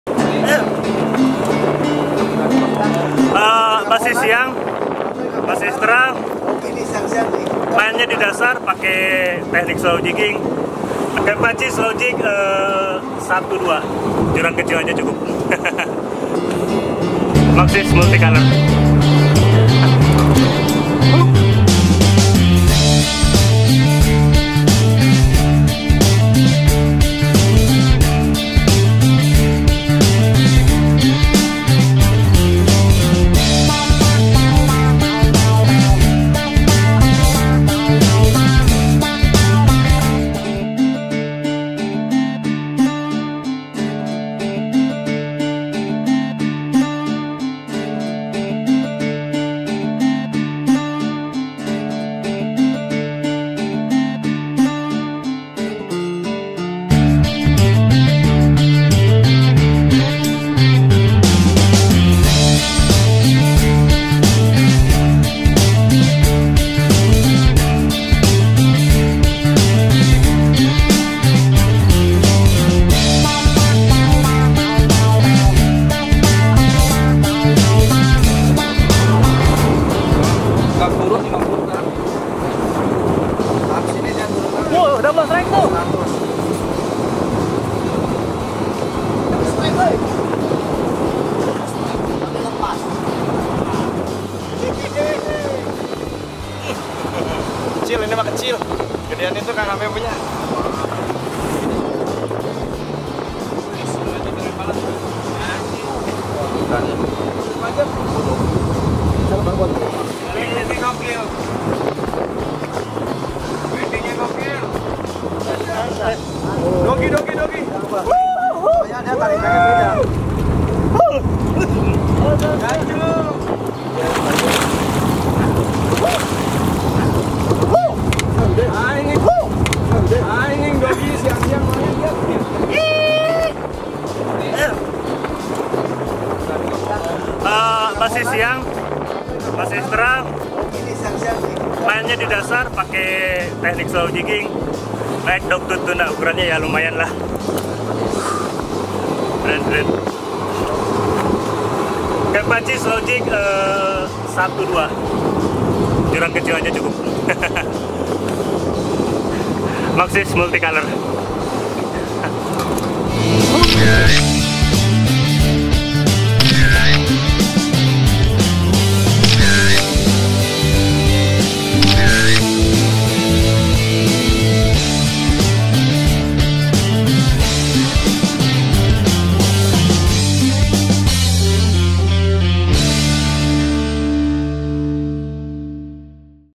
A Great Strike with Duraking Maxis
Salah satu momen terbaik setiap Angler adalah ketika strike yang didapat berakhir dengan kemenangan. Begitupun dengan Harry Buana, adu otot-nya yang panjang dengan predator di lautan Raja Ampat berakhir dengan sangat membanggakan.